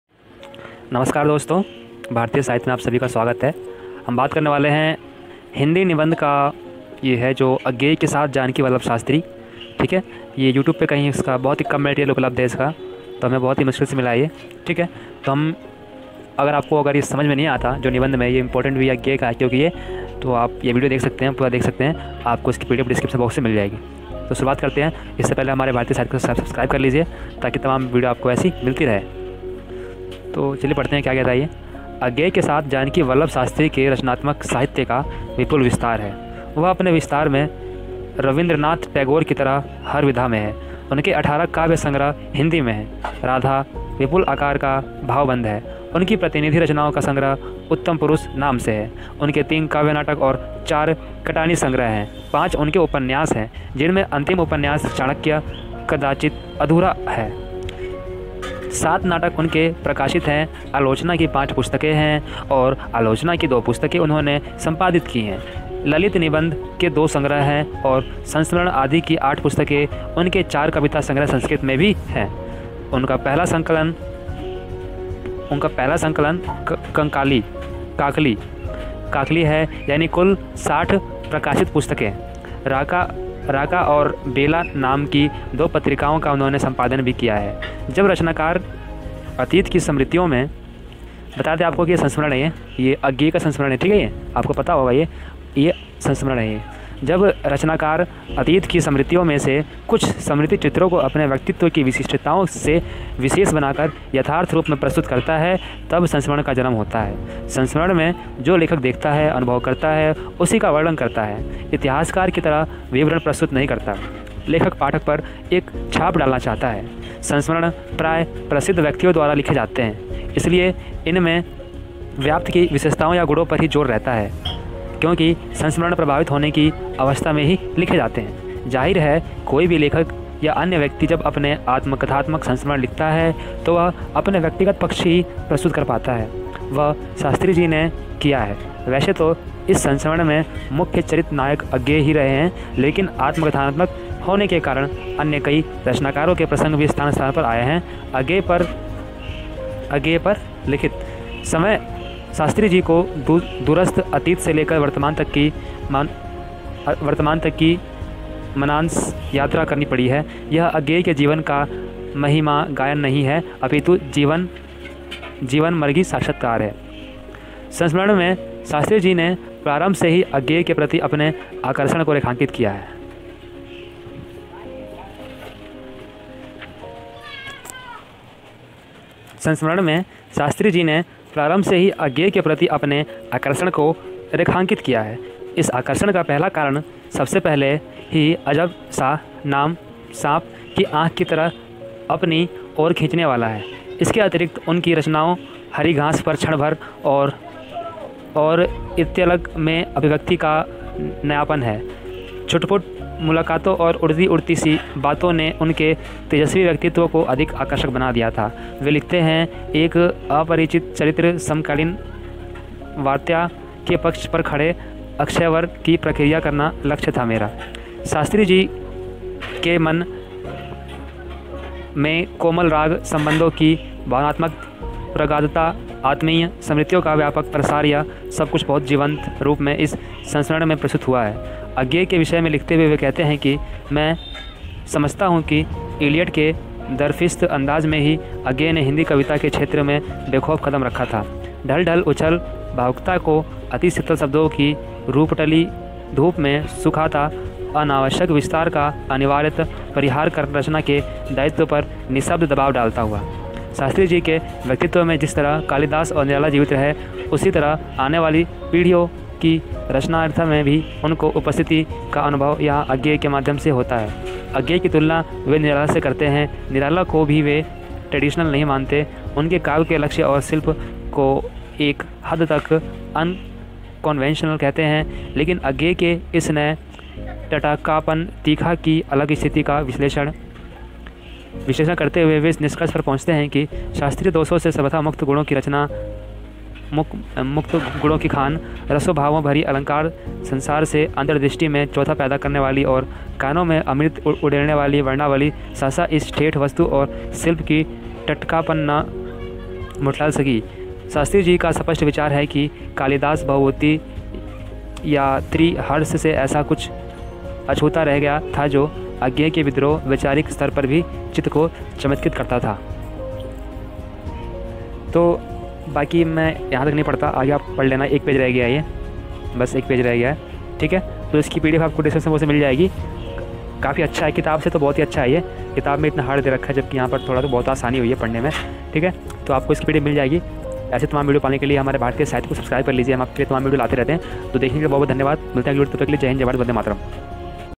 Agey ke sath aacharya janki vallabh shastri sansmaran ki samiksha//आगे के साथ आचार्य जानकी वल्लभ..
pdf 👇 link

 के साथ आचार्य जानकी वल्लभ शास्त्री की आलोचना आप इस वीडियो में देखेंगे आपको इसकी पीडीएफ भी उपलब्ध करा दी जाएगी अगर नहीं है तो कमेंट कर देना आपको इसकी वीडियो भी मिल जाएगी बुक में थोड़ा कठिन दिया हुआ है और इसकी यूट्यूब पर बहुत ही कम वीडियो क्लब दे तो आप कमेंट करके वीडियो प्ले सकते हैं ठीक है इसमें थोड़ा बहुत आसान भाषा में है जो आपको बहुत ही मदद करेगा तो दिखने लगा धन्यवाद।।।